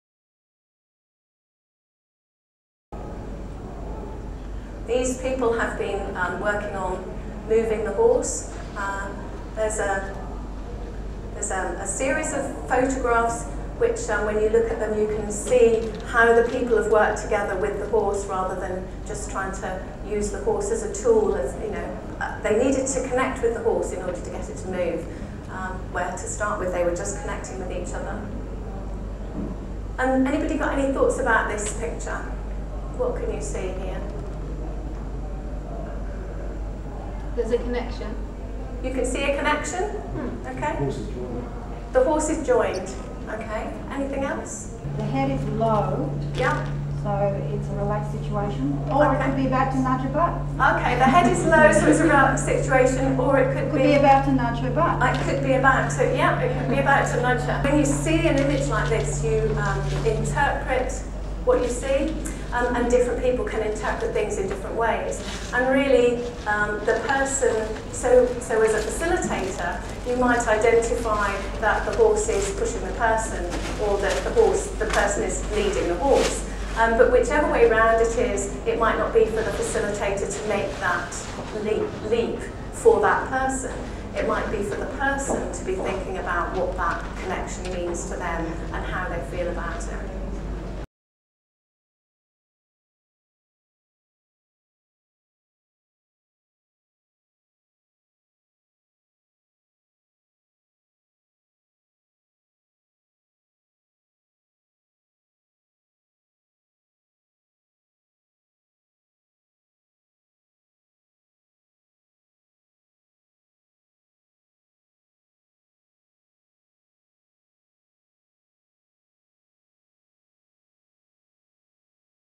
Video 7 EAQ® at Equitana, Sydney 2013 What happens in an equine assisted learning session?
This is video number 7 taken from a lecture about equine assisted learning given by EAQ founder, author and international educator, Tricia Day at Equitana, Sydney 2013.
Tricia talks about the role of the equine assisted learning facilitator and how it's not for the facilitator to decide what's happening in the interactions between horses and humans.